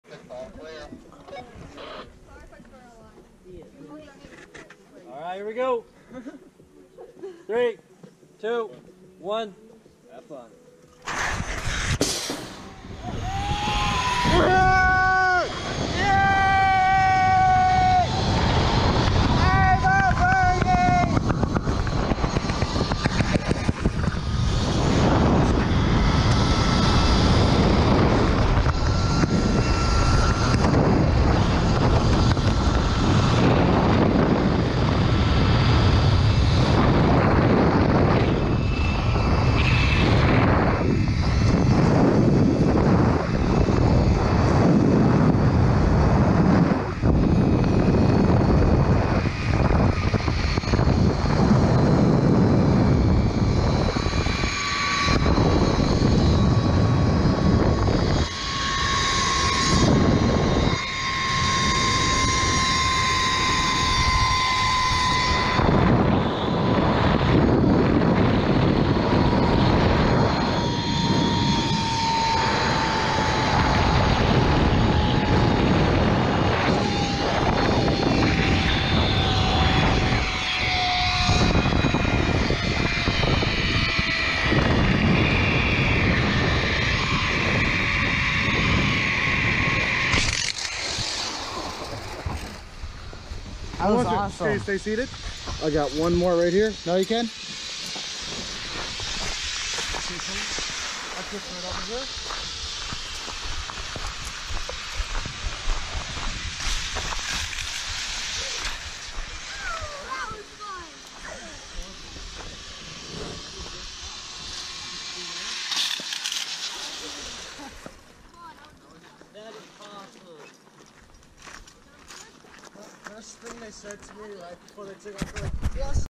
Nate on a ZipLine in Hoonah.
Nate on the Zip Line in Hoonah, Alaska at Icy Strait Point (ZipRider). June 22, 2018